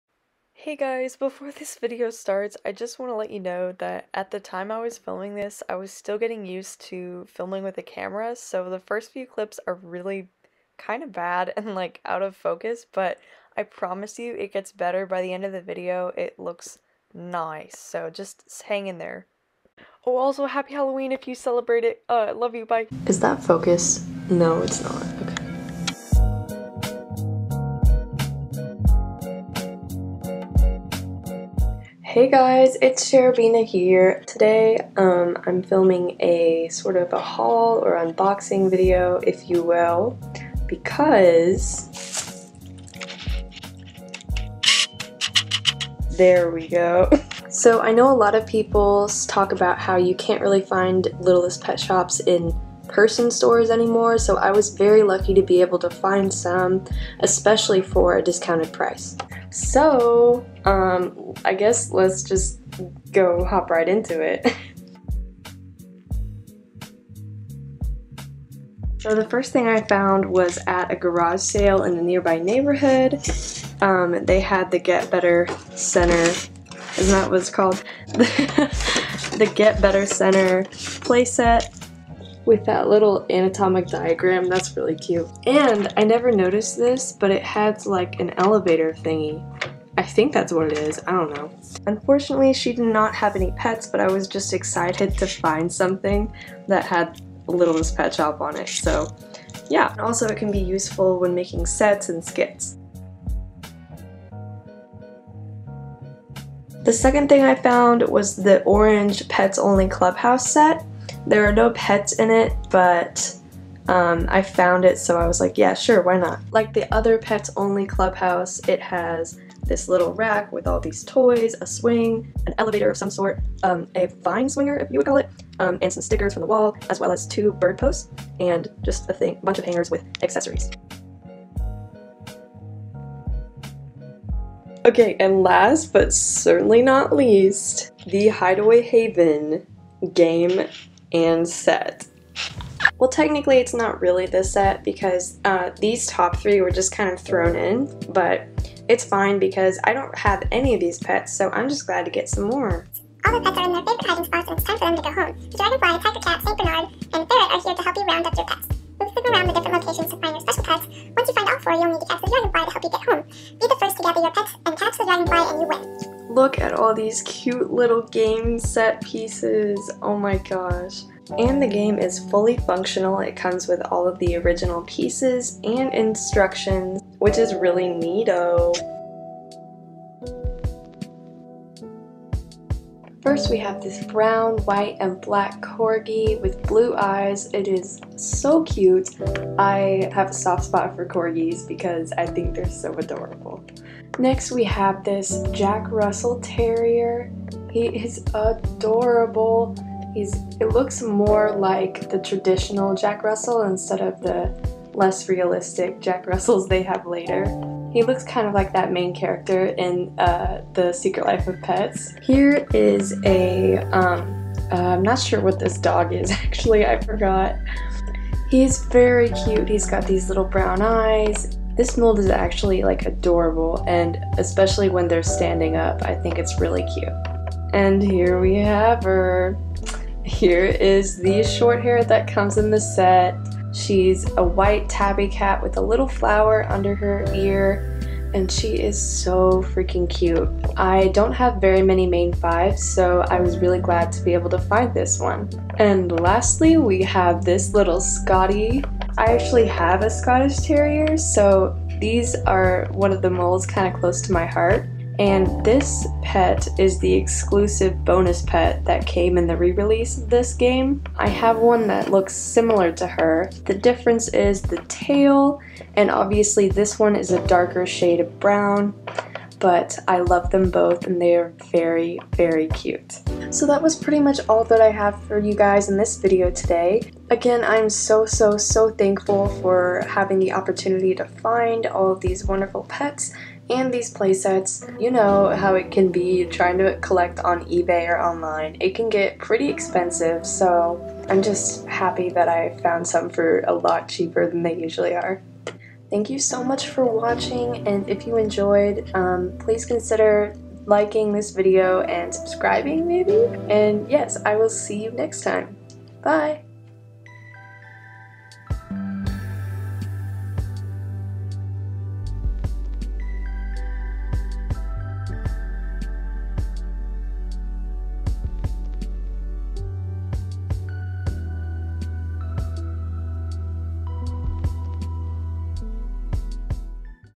lps || flea market finds with chère!
hi lovely!
in this video, i'll be showing you guys some lps i've found by shopping locally! i've really enjoyed making this video, let me know if this would be an interesting series? also, lmk any other videos you would want to see! anyways, happy halloween everyone! tomorrow is november 1st and that means christmas season...

(and thanksgiving too i guess.)

social media !
ig ; @cherubinalps
lpsa ; cherubina

until we hit the heavens...